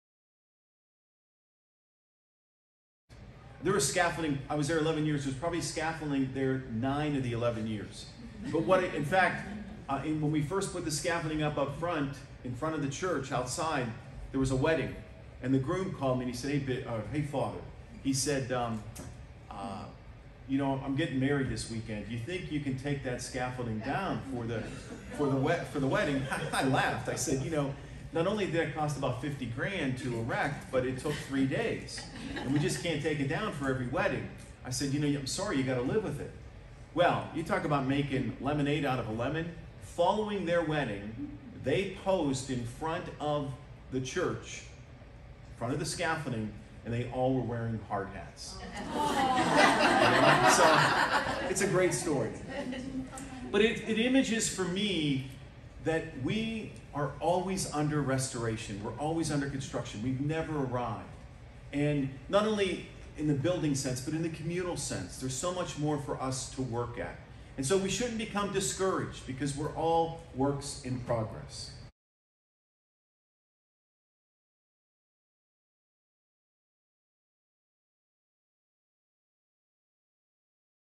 Bishop Bonnar scaffolding story
The Most Rev. David J.  Bonnar, Bishop of Youngstown, shares a short story about scaffolding at a parish in which he served as pastor.